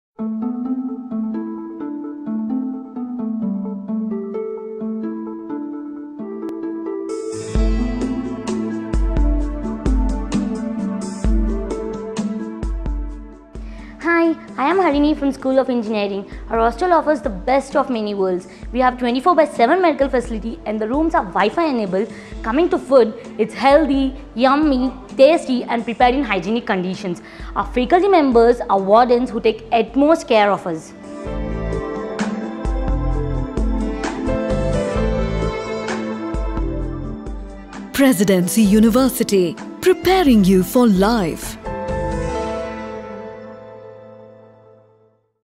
Student Testimonials - Hostels @ Presidency University, Bangalore
Harini on Hostel Facilities @ Presidency University:
Presidency University provides separate hostel facilities for Girls and Boys, which are adjacent to the educational campus. The hostels are managed by the College administration. These hostels offer modern amenities and are neat and well-maintained.